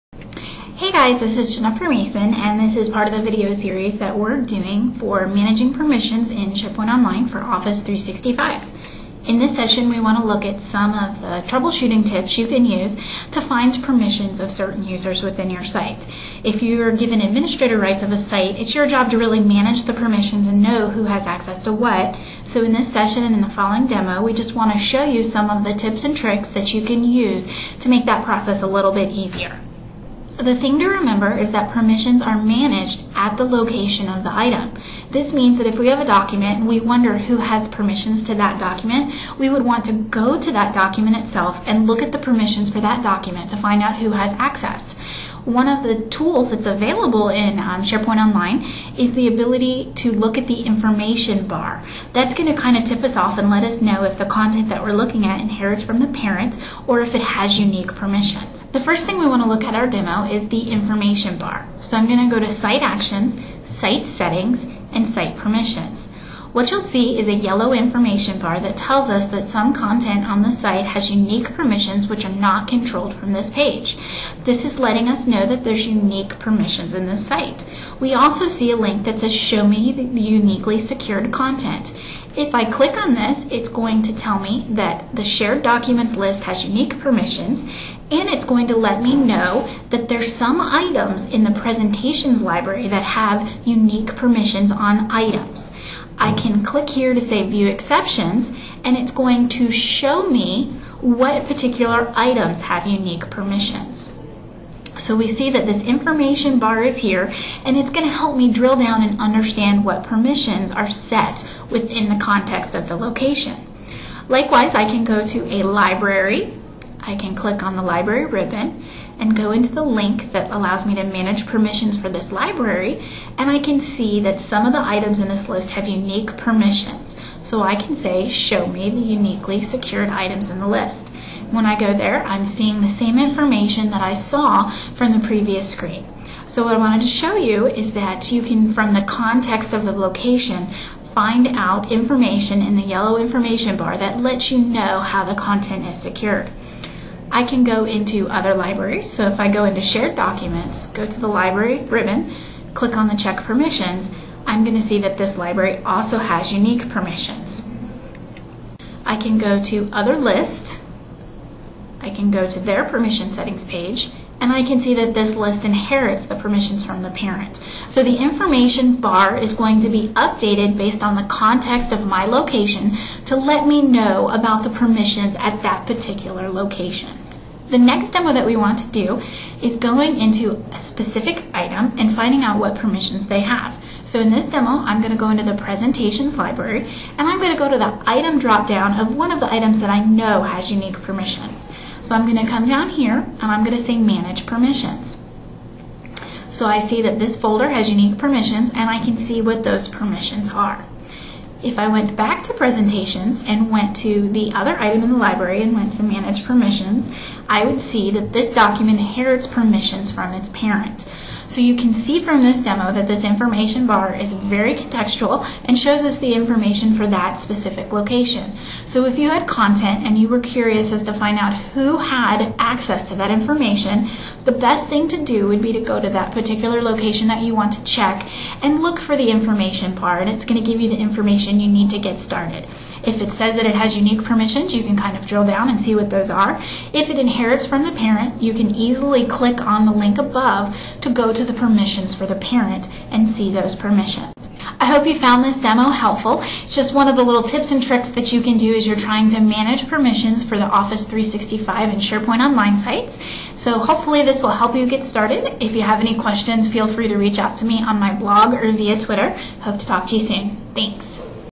SharePoint Permissions: Basic Troubleshooting
This installment in our series on SharePoint permissions looks at tips and tricks for basic troubleshooting. Watching these videos offer quick and easy methods to understand how the permissions on your site have been configured.